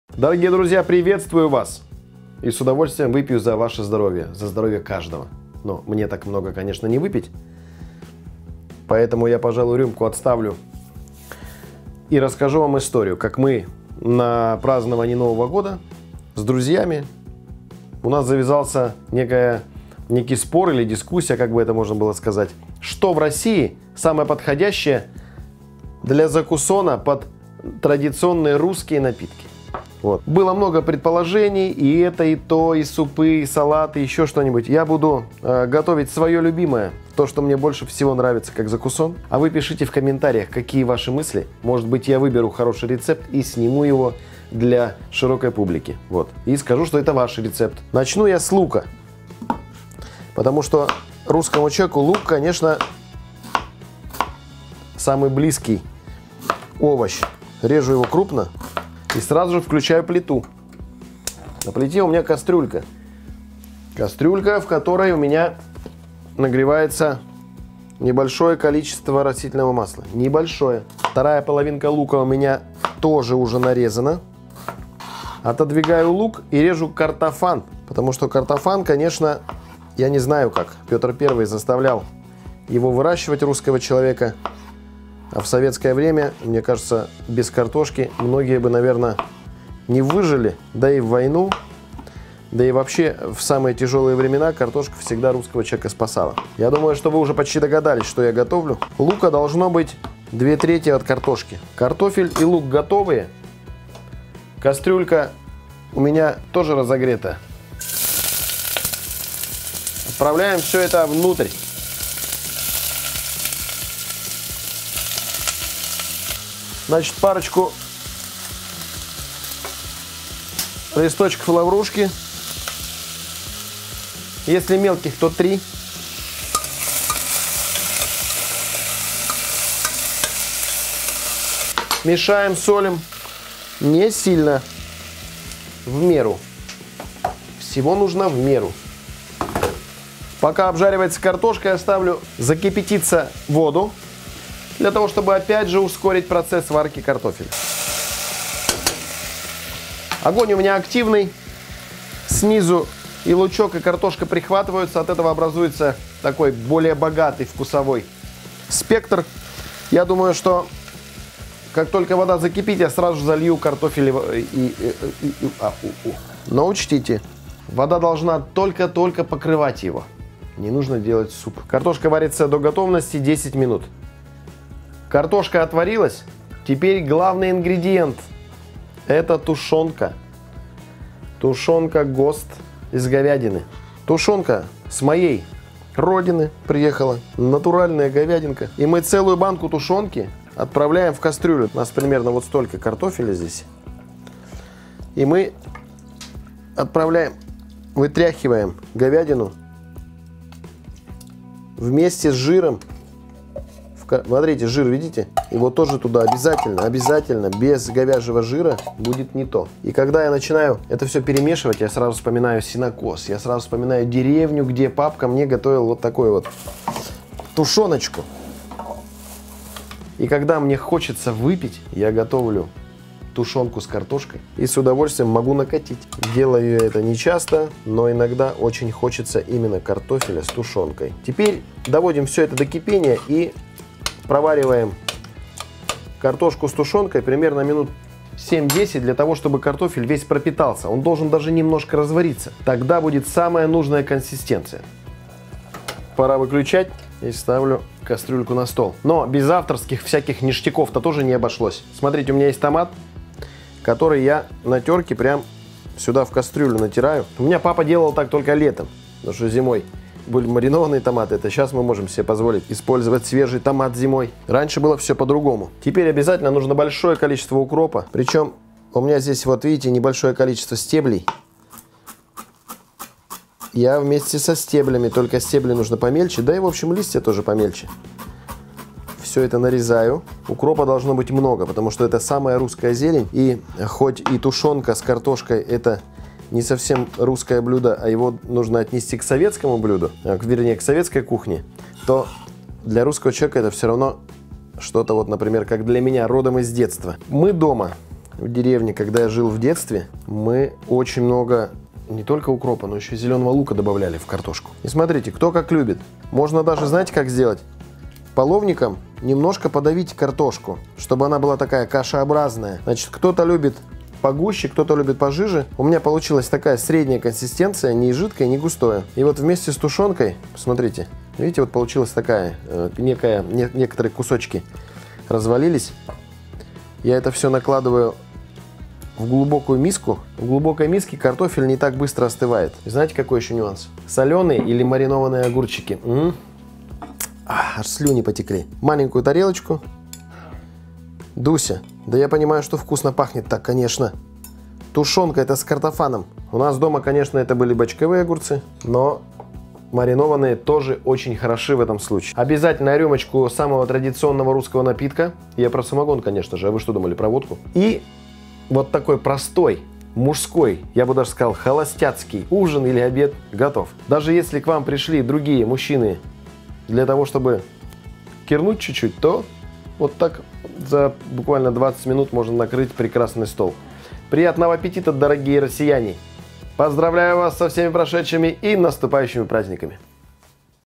Любимое блюдо для закуски самогона. Простой рецепт. Русские закуски.
Мужики!!! С Праздником Вас!!! С 23 февраля!!! Сегодня на столе все просто и душевно. Самогончик и очень легкая в приготовлении, но безумно вкусная закуска к горячительных напитков.